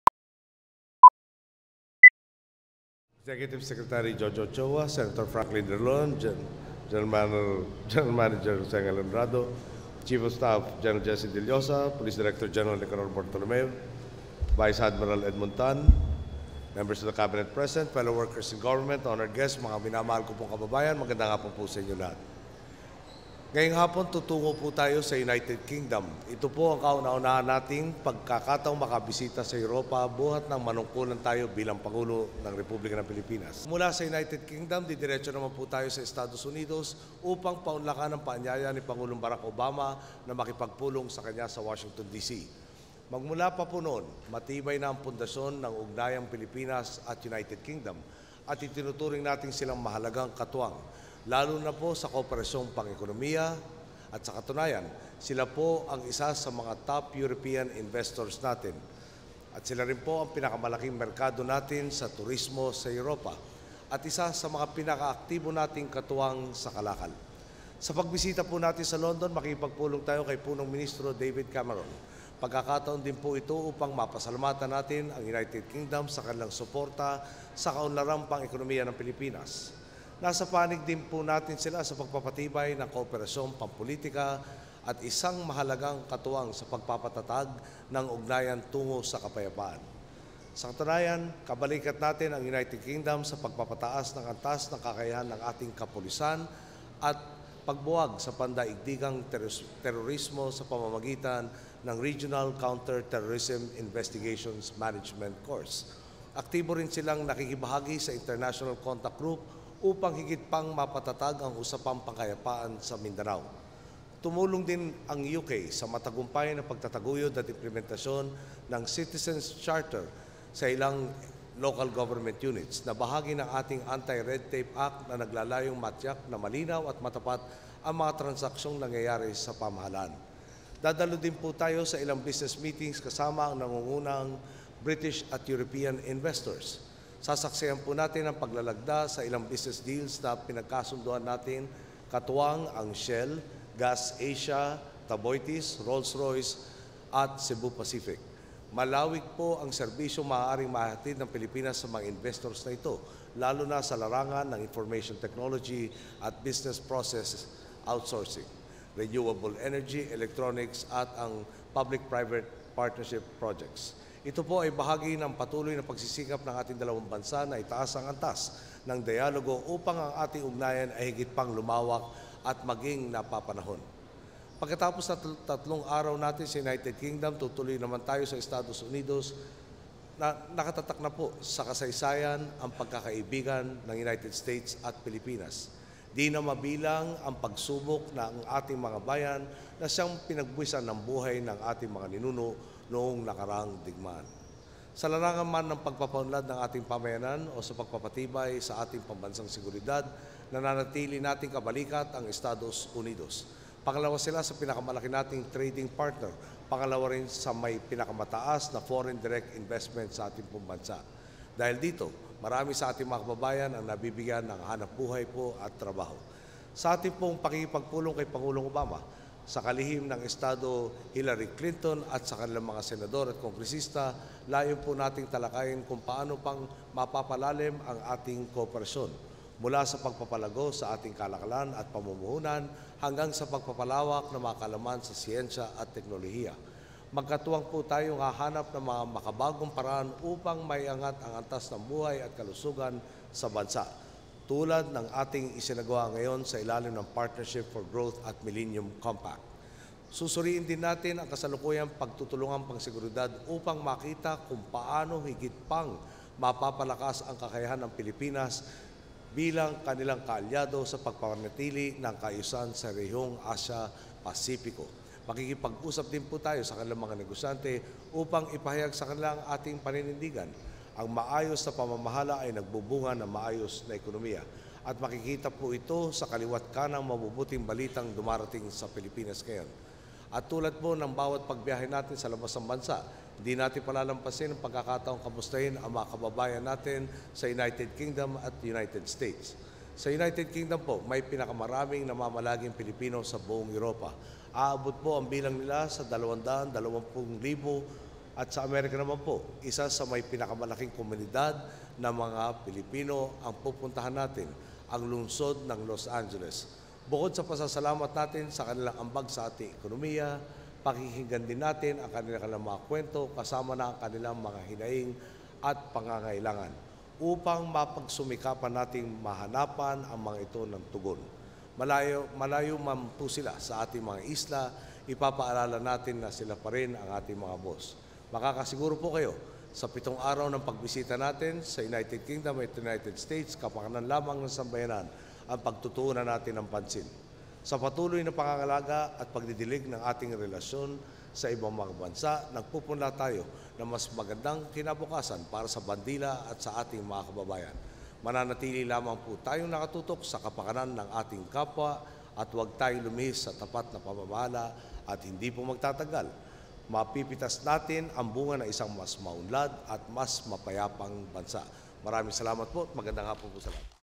Departure for United Kingdom and the USA (Speech) - 6/4/2012
NAIA Terminal 2, Pasay City
04 June 2012
President Benigno S. Aquino III left for his first official visit to the United Kingdom (June 4-6) and a working visit to the United States (June 6-10).

In London, the President is set to meet Prime Minister David Cameron and thank him in supporting Philippine economic development. He will also hold business meetings with leading British and European investors.

"Malawig po ang serbisyong maaaring maihatid ng Pilipinas sa mga investors na ito, lalo na sa larangan ng information technology at business process outsourcing, renewable energy, electronics at PPP projects," the President said in his departure speech.

For his four-day working visit to Washington D.C. and Los Angeles, California, President Aquino will hold meetings with US President Barack Obama, US Secretary of State Hilary Clinton, business leaders and other government officials to discuss ways in deepening bilateral cooperation.

"Makakasiguro po kayo: sa pitong araw na pagbisita natin sa UK at US, kapakanan lamang ng sambayan ang pagtutuunan natin ng pansin," he remarked.

Eight Cabinet members from the Departments of Foreign Affairs, Finance, Defense, Trade and Industry, Tourism, Transportation and Communications, Energy and the Presidential Communications Development and Strategic Planning Office joined the President onboard.